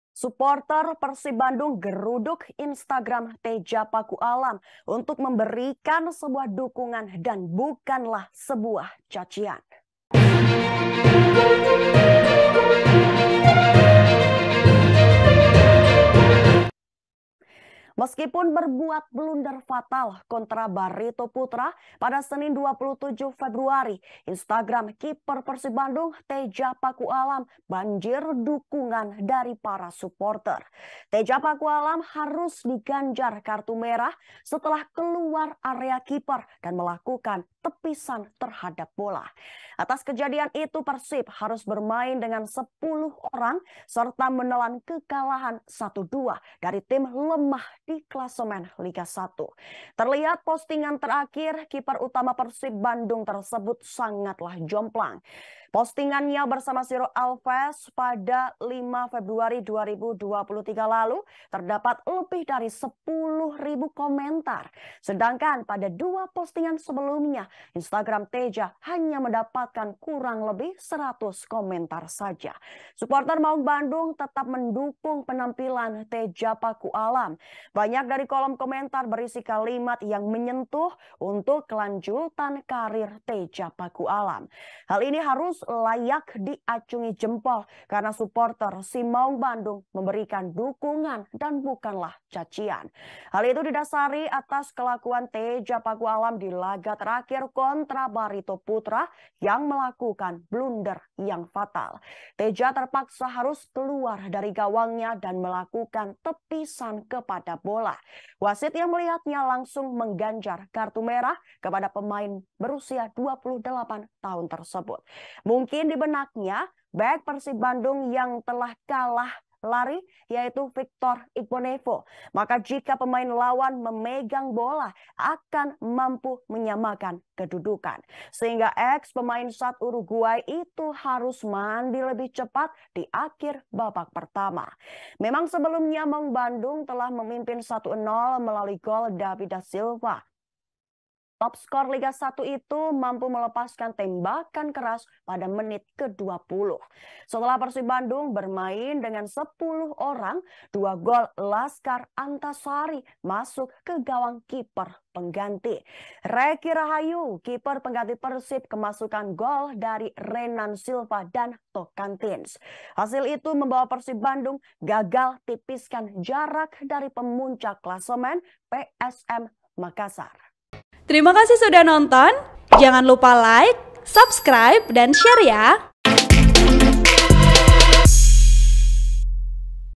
Bobotoh Banjiri Postingan Kiper Persib Bandung Teja Paku Alam, Beri Dukungan
TRIBUNJABAR VIDEO -  Bobotoh memberi dukungan kepada kiper Persib Bandung, Teja Paku Alam.

Teja Paku Alam melakukan blunder saat Persib Bandung melawan Barito Putera, Senin (27/2/2023).

Dalam laga itu Persib Bandung mengalami kekalahan di kandang Barito Putera, Stadion Demang Lehman, Martapura, dengan skor 1-2.

Teja Paku Alam diganjar wasit kartu merah setelah keluar area kiper dan melakukan tepisan terhadap bola. (*)

Editor: Januar Pribadi Hamel | VE: dicky fdj